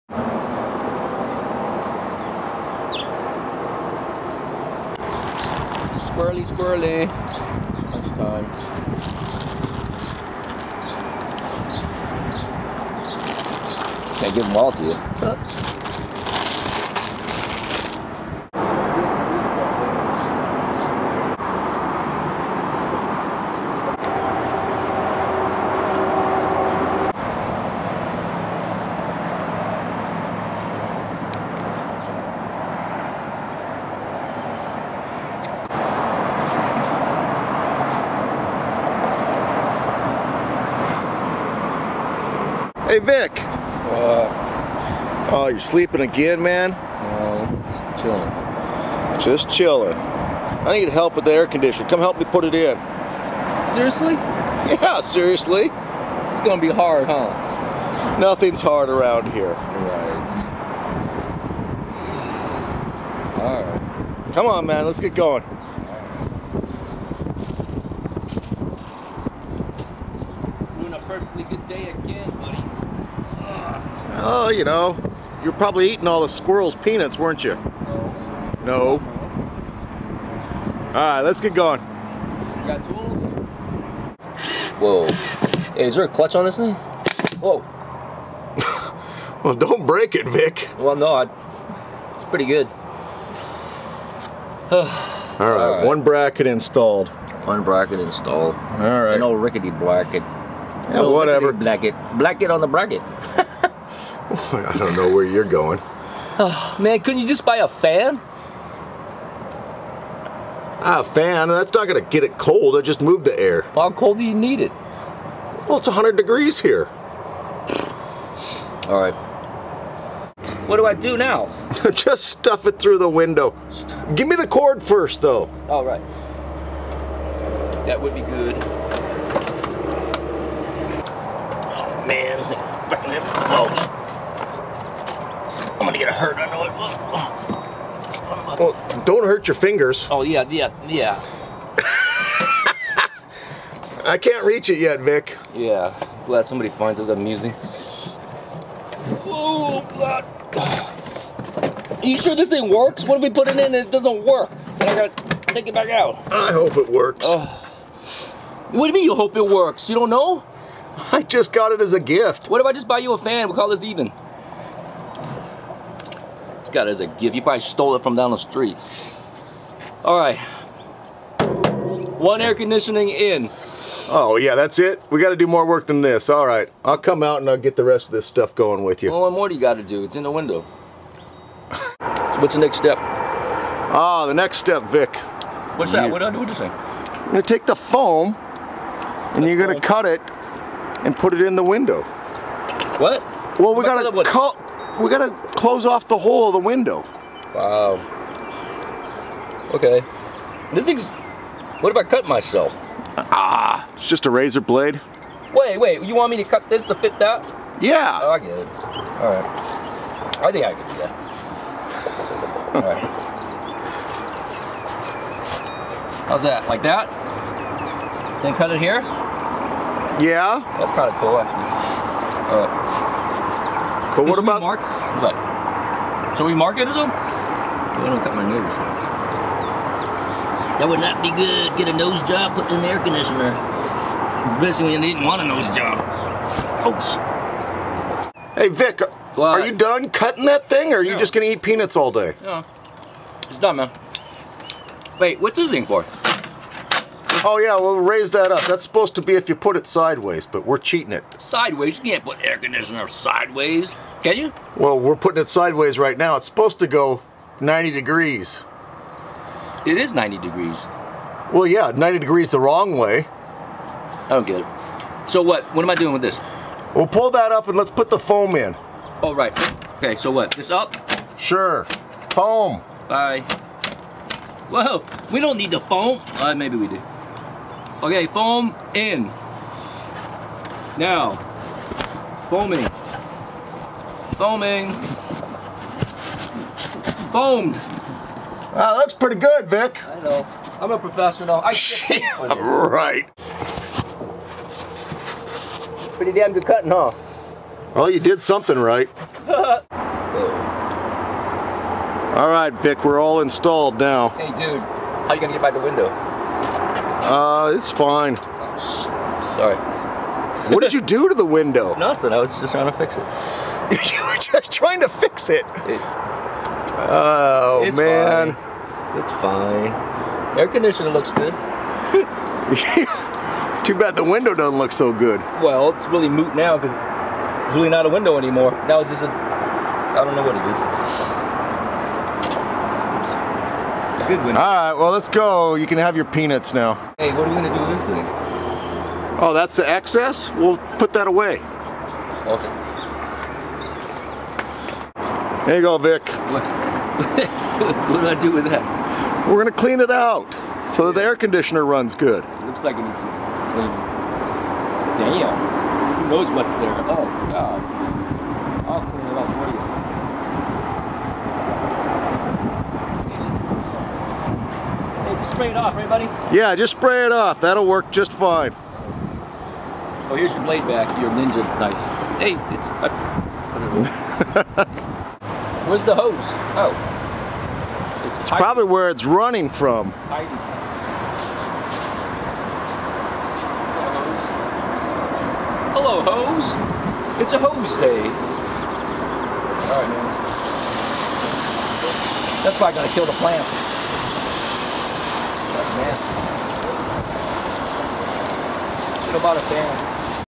Vic Works 2
I talked Vic into helping me install a new air conditioner.